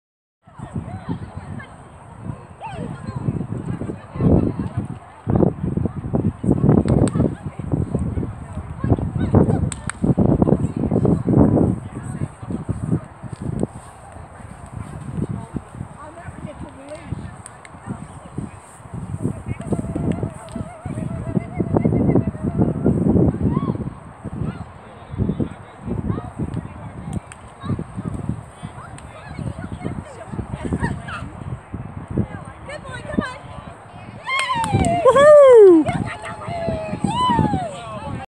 Rob OpenJWW DASH 8 8 15
Rob's Ex Standard run at the DASH AKC agility trial on Saturday, 8/8/15.